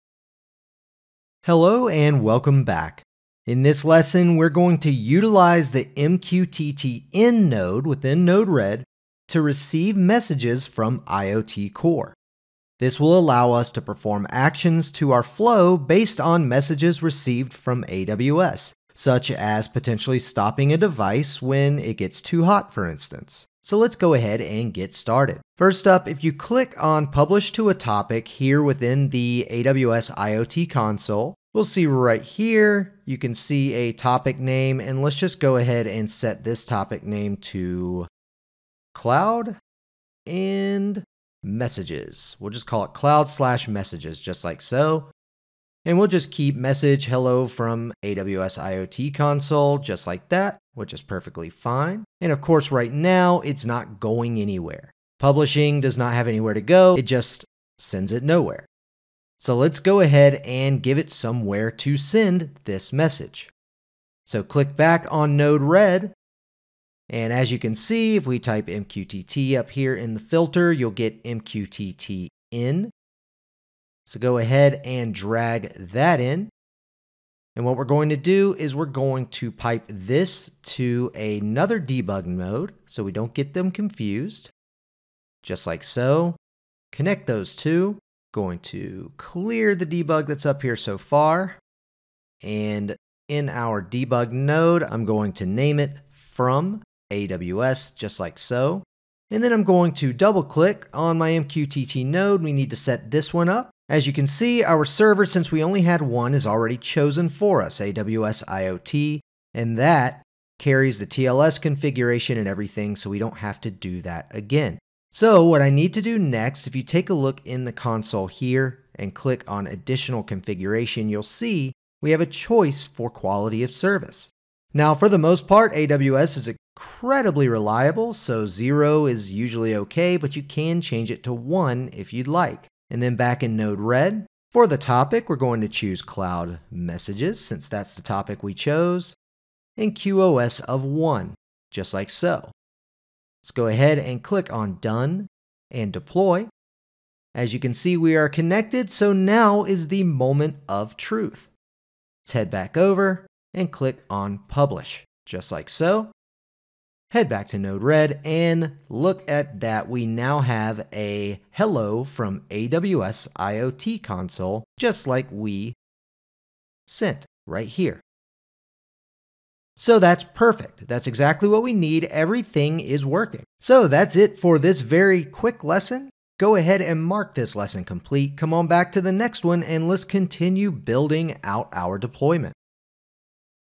Receiving MQTT Messages in Node-RED
In this video, we'll use the MQTT In Node to receive MQTT messages from AWS IoT Core.